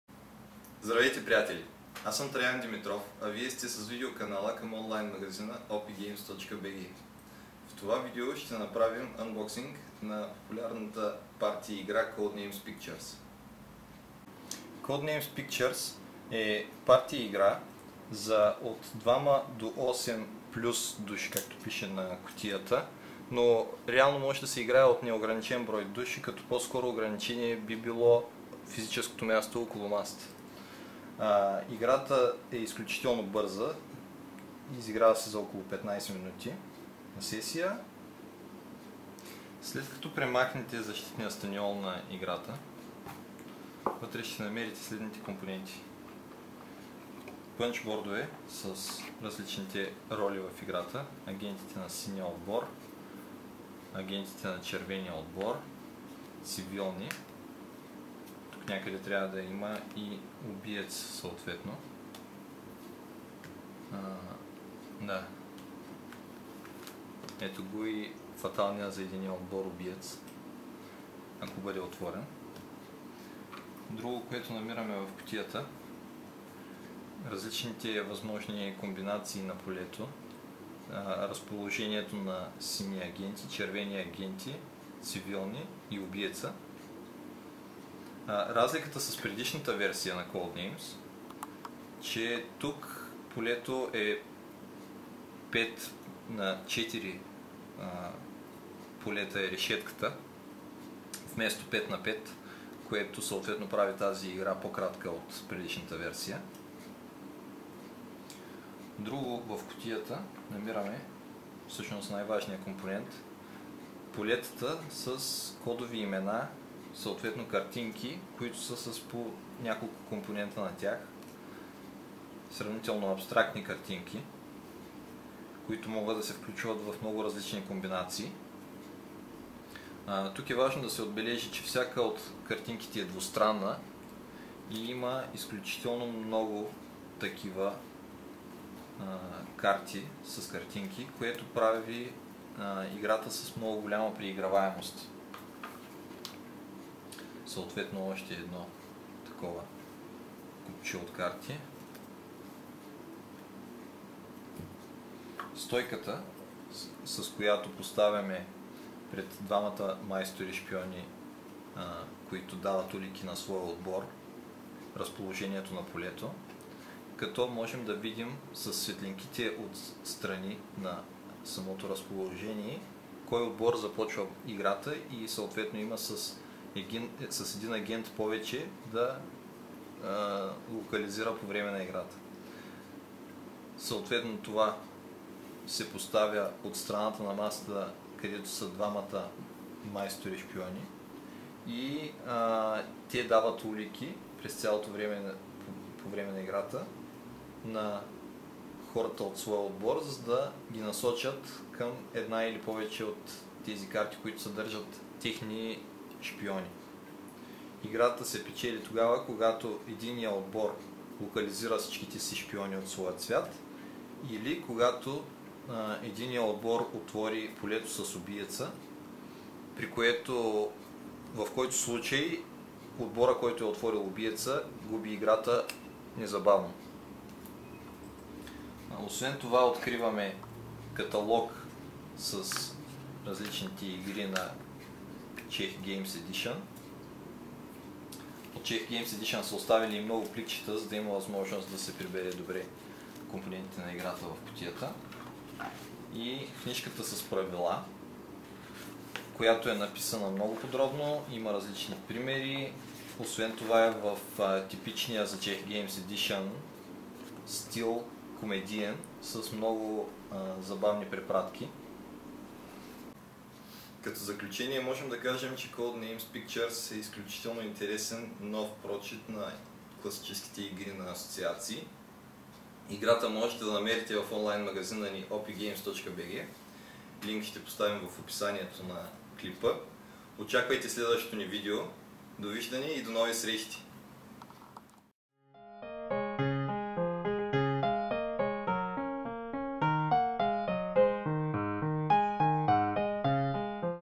Ънбоксинг на играта Codenames Pictures
Codenames Pictures можете да закупите заедно с бонус редки промо плочки към играта от

Описание:
В този епизод Траян Димитров показва какво има в кутията на популярната настолна парти игра на асоциации Codenames Pictures от издателя Czech Games Edition.

- Влезте в света на настолните игри с OPGames.BG!